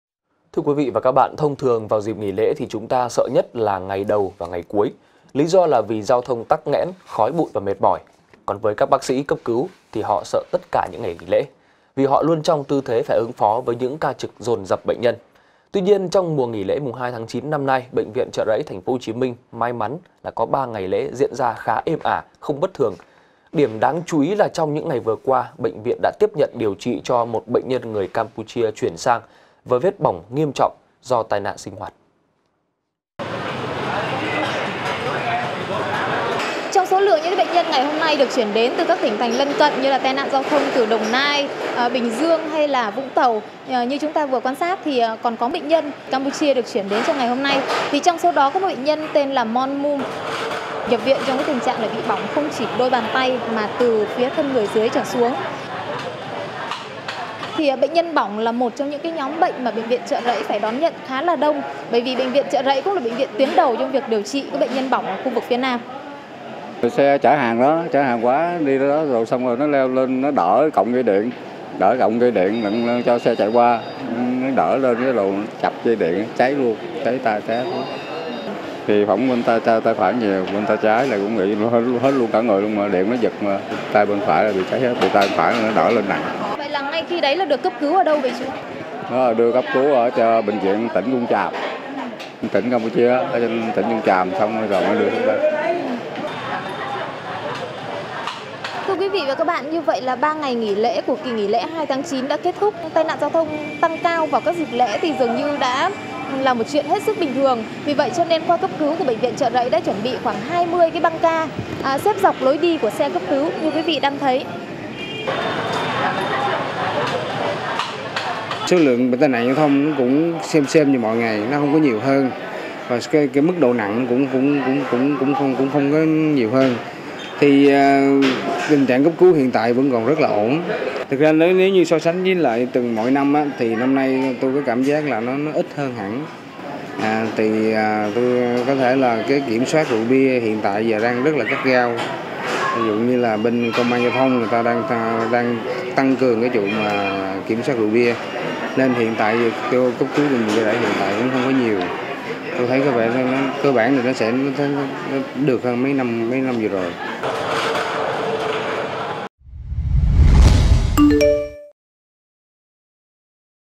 VTC14 | Dùng tay gạt dây điện, lơ xe bỏng toàn thân
(VTC14) - Mùa lễ 2/9 năm nay BV Chợ Rẫy TP HCM may mắn có 3 ngày lễ diễn ra khá êm ả, không bất thường. Điểm đáng chú ý là bệnh viện đã tiếp nhận điều trị cho 1 bệnh nhân người Campuchia chuyển sang, với vết bỏng nghiêm trọng do tai nạn sinh hoạt.